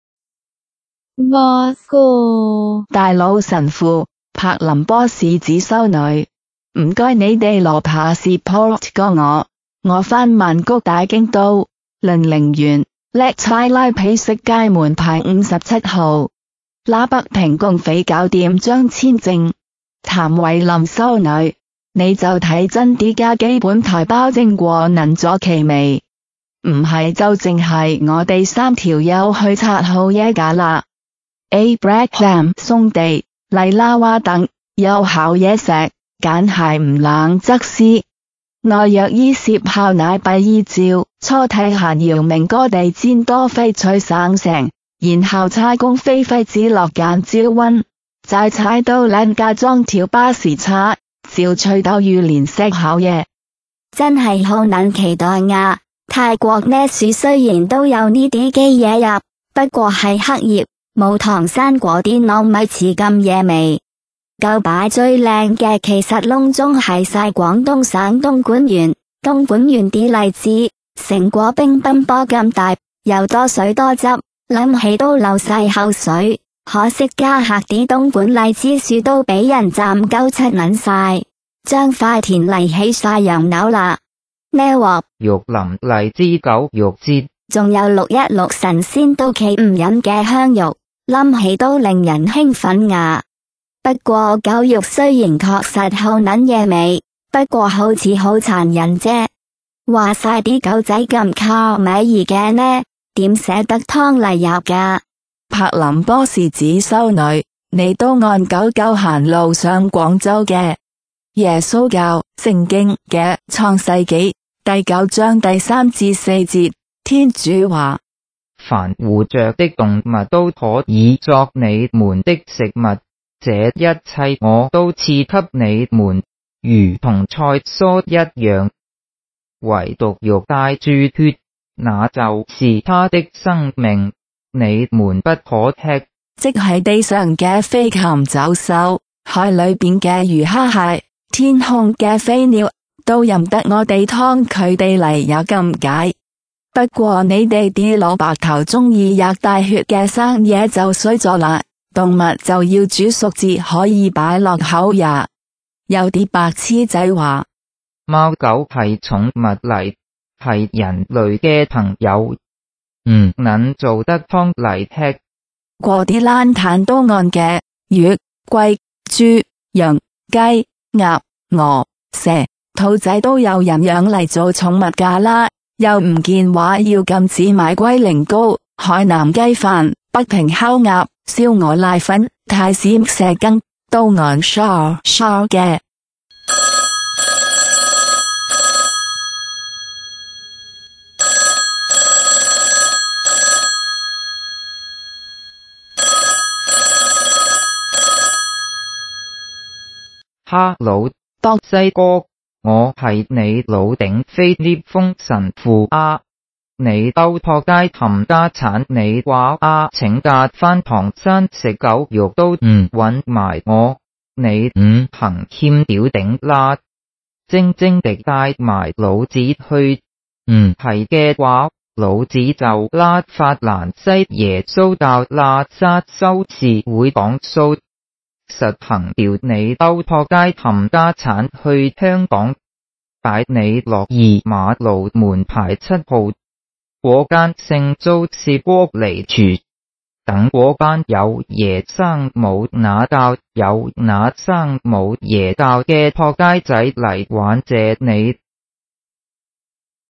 dog cuisine and lychee fruit in mainland China
In bosham channel, our virtual roles Reverend บอสโก (扑西哥神父), Mr. อับราฮัม (啞巴喊), Sister Busuko Berurin (伯林ボス子 (柏林波時子)) and Sister Wei-lin Abra Ham (譚瑋琳) plan to visit mainland China for its "Lychee and Dog Meat Festival in Yuk Lam county (玉林荔枝狗肉節)" on 21st June, 2016, Tuesday.

Yuk Lam county  (玉林縣) belongs to Kiangsi province (廣西省), it is closed to Kwangchowan (ZHA), the post French colony, nowadays the Peking communist bandits call this place as "Tsankiang (湛江)".

Let us to review the Bible for eating dog meat, it is section 3 and 4, chapter 9 from the "Genesis (創世紀)":

Every living thing that moves will be yours to eat, no less than the foliage of the plants. I give you everything, with this exception: you must not eat flesh with life, that is to say blood, in it.

凡活著的動物（every living thing that moves），都可以作你們的食物（will be yours to eat），這一切我都賜給你們如同菜蔬（the foliage of the plants）一樣。惟獨肉帶著血（flesh with life (blood in it)）、那就是牠的生命、你們不可喫。

Thus, from a Catholic point of view, we can feel free to enjoy the delicious dog and cat cuisine, but don't eat cold raw food, such as Japanese sashimi (刺身).